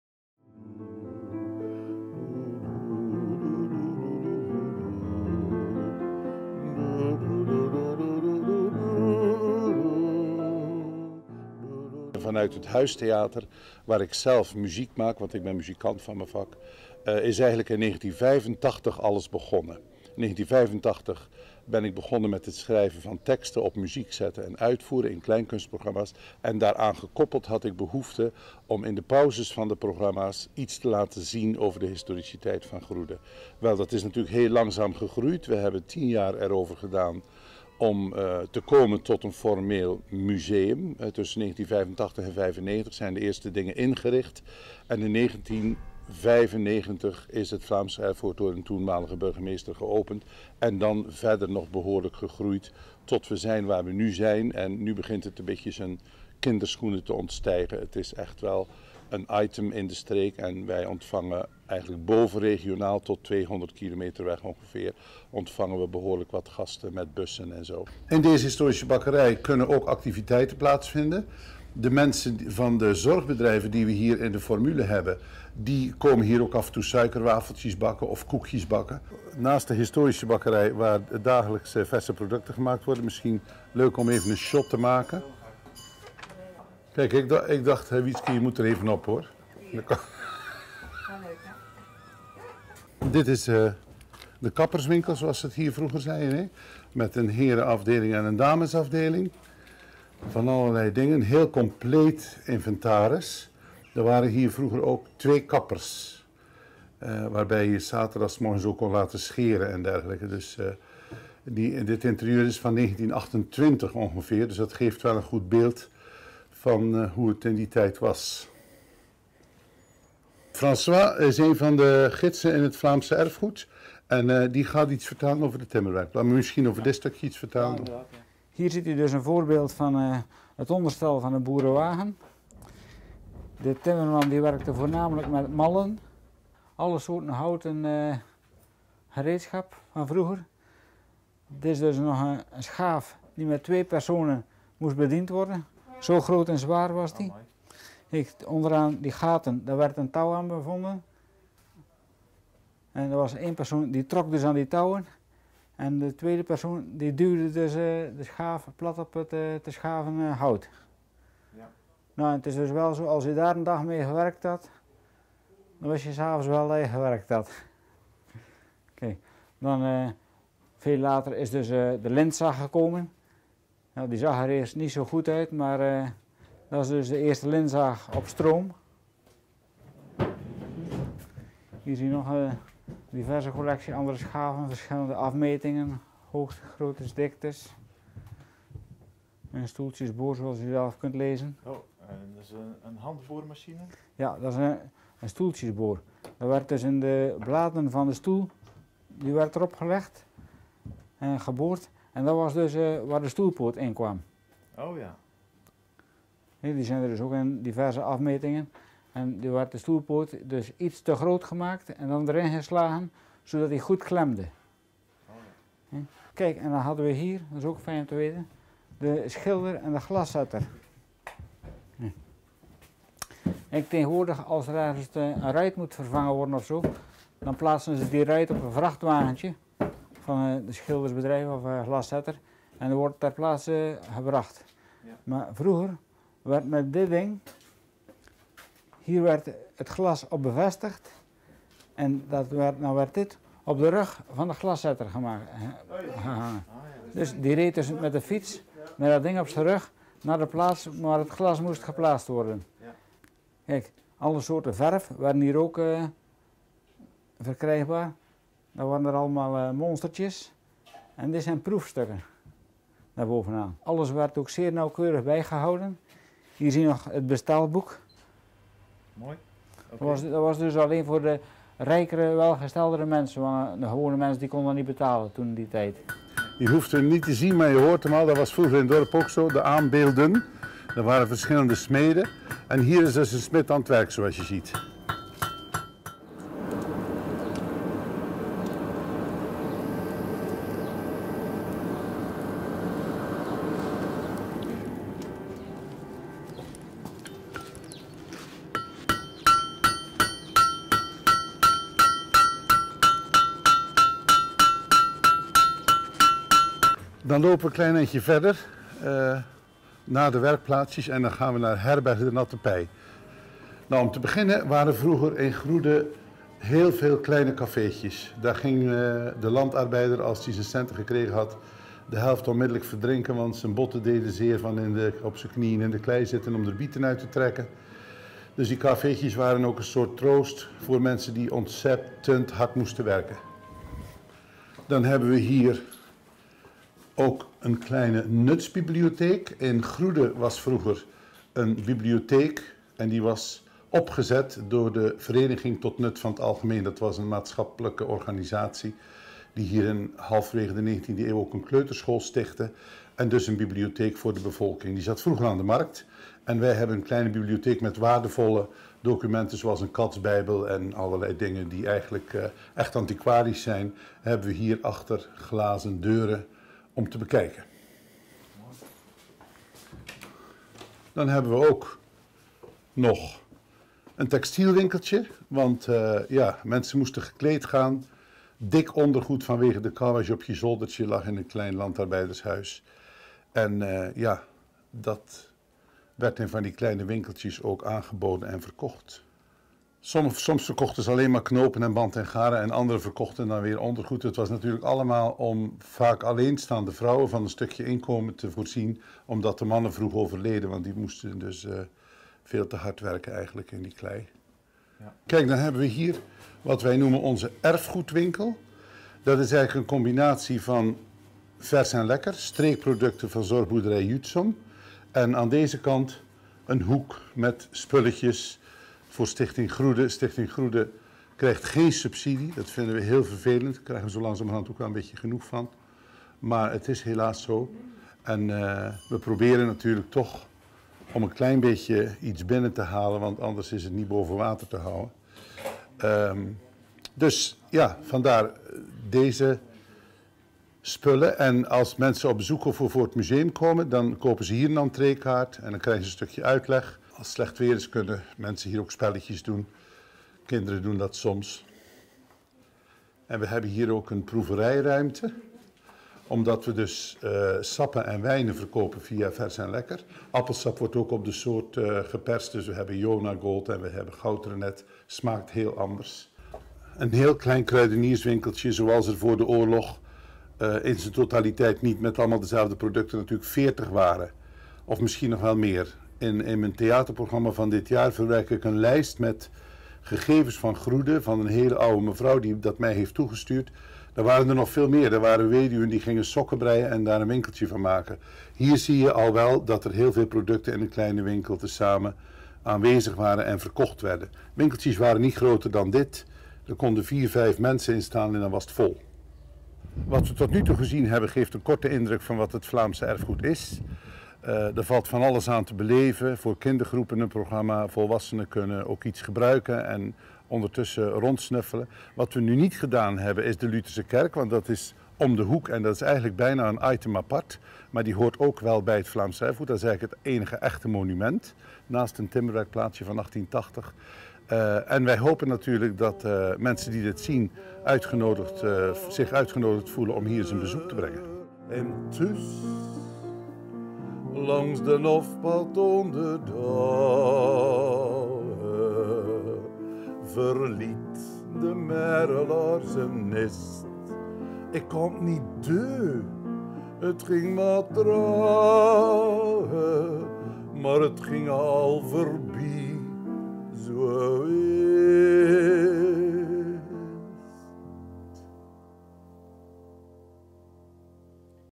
Slijkstraat Groede
In de pittoreske Slijkstraat van het Vlaemsche Erfgoed maak je kennis met het ambachtelijk leven van het Groede uit vroeger tijden.